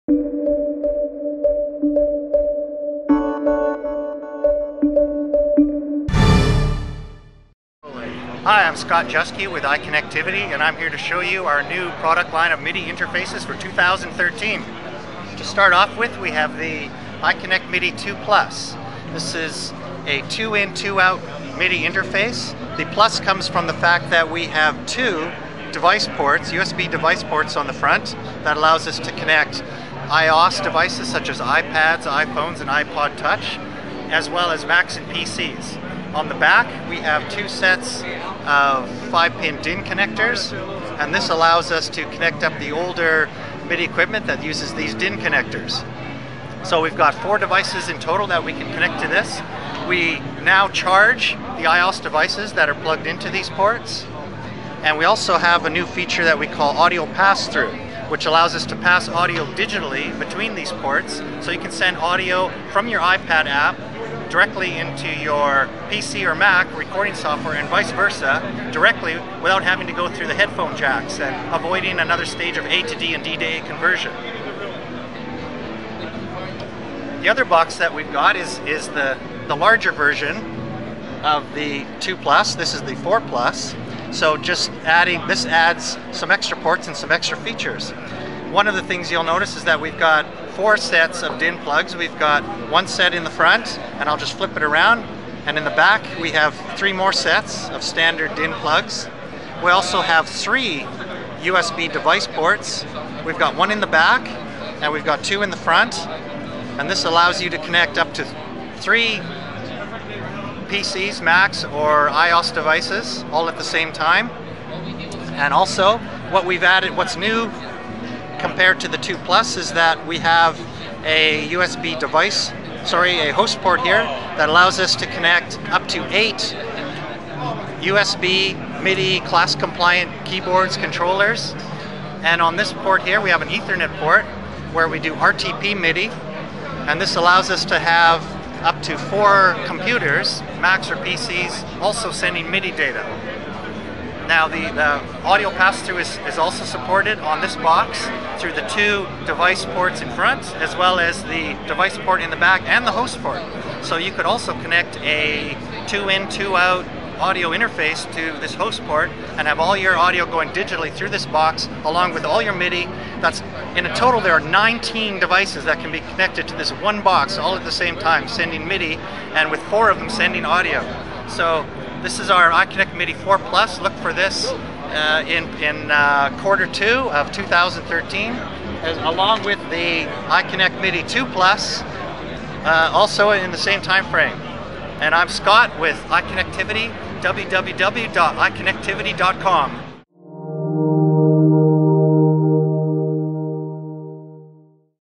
Winter NAMM 2013 iConnectivity iConnectMIDI interfaces
These two models provide interfacing for iOS devices, Mac, and PC via USB.